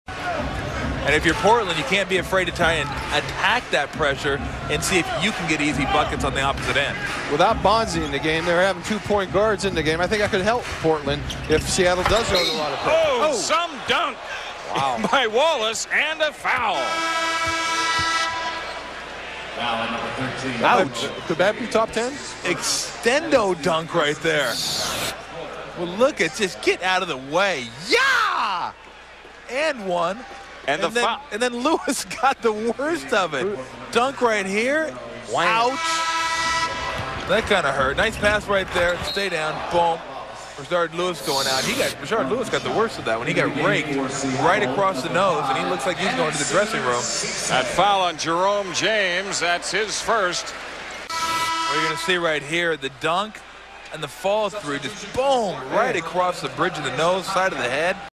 Rasheed POSTERIZES Jerome James (11/7/03)
Rasheed Wallace's Poster jam in Seattle on Nov 7, 2003